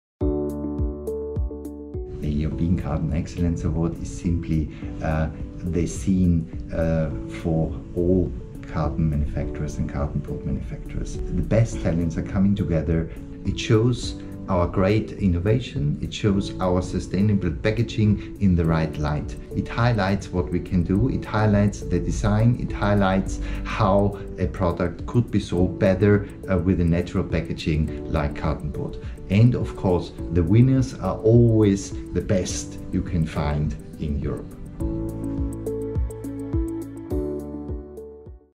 Horst Bittermann about the European Carton Excellence Award
Pro Carton is the European Association of Carton and Cartonboard Manufacturers. They hold the European Carton Excellence Award, the Pro Carton Young Designers Award as well as several design awards on a national basis.